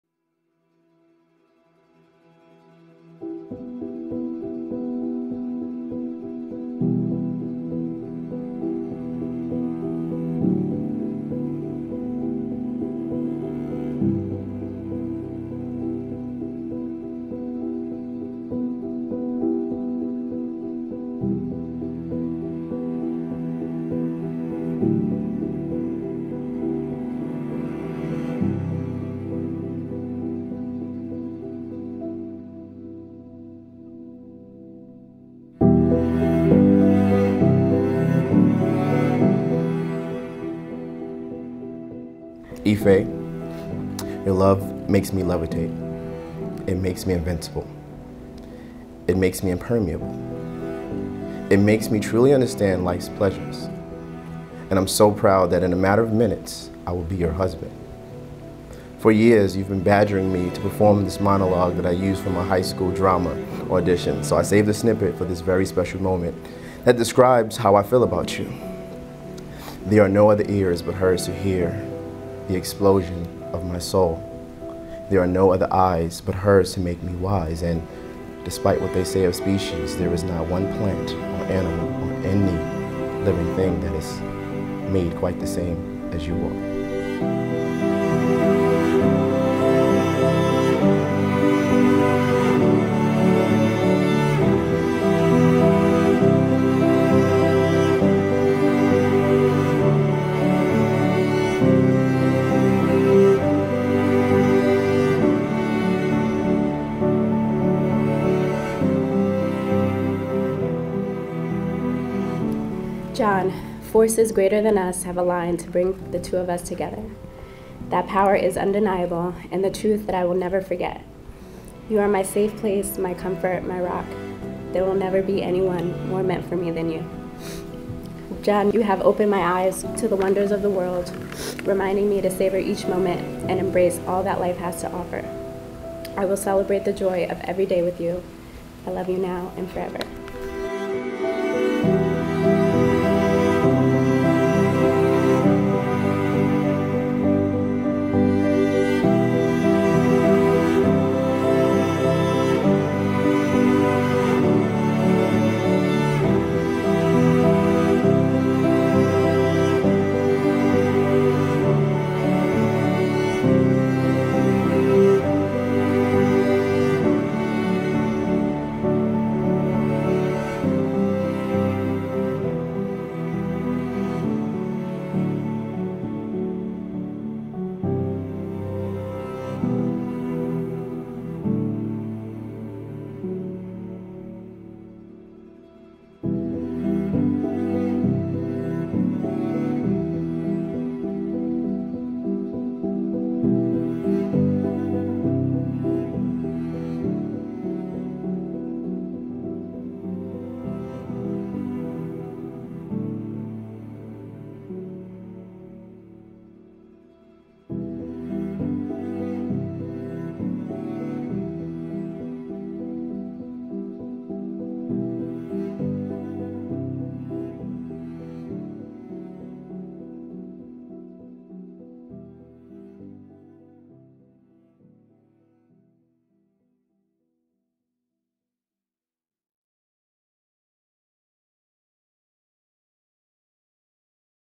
Emotional Wedding Video at Hummingbird Nest Ranch
Emotional Wedding Video at Hummingbird Nest Ranch. Hummingbird Nest Ranch is a gorgeous wedding venue, one of the top around Los Angeles Area.

IG cinestoryfilms